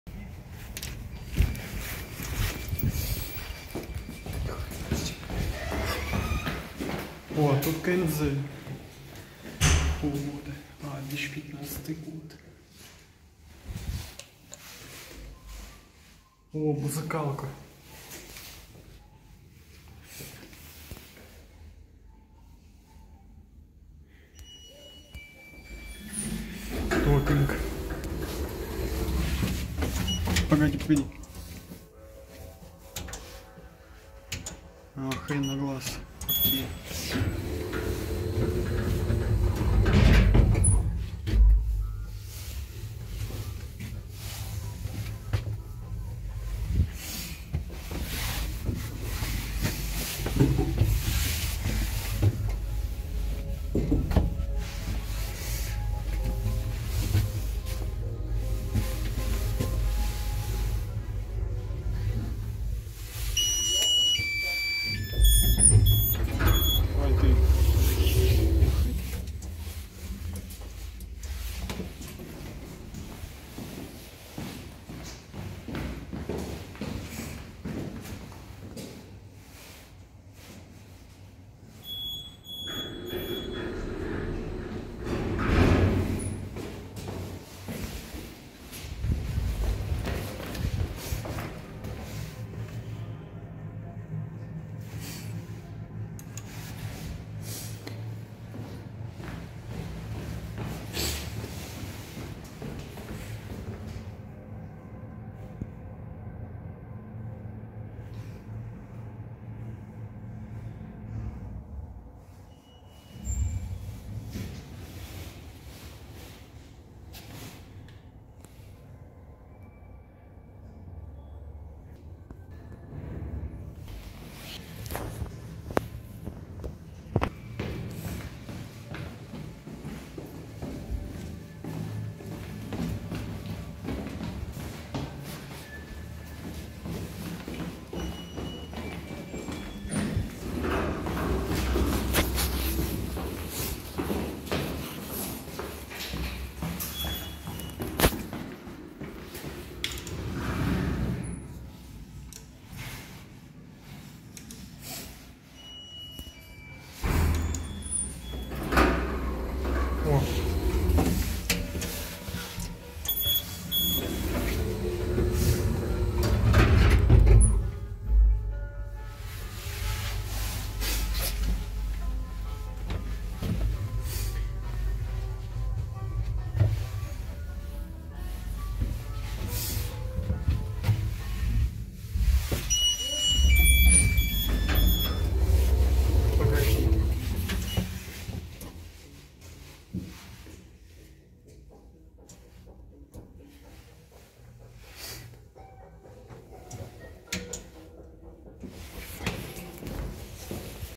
ЛИФТ КМЗ-(2015 Г.в) Q=400/kg V=1m/s Г. Королев Россия
Г. Королев проспект Королёва, 11В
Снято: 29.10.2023
В Видео Обзоре Участвовал Данил Брант

Марка Лифтов: КМЗ

Модель: Карачаровский Механический Завод

Грузоподъемность Q=400/kg

Вместимость 5.чел

Скорость V=1m/s